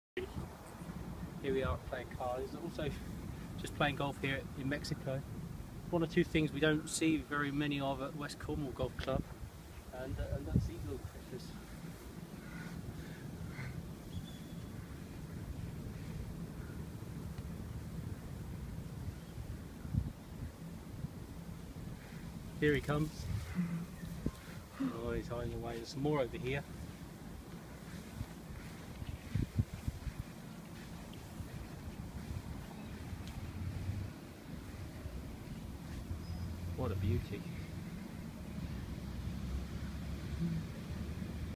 What you dont see playing golf at West Cornwall but you do in Mexico
Description Playacar Golf Club in Mexico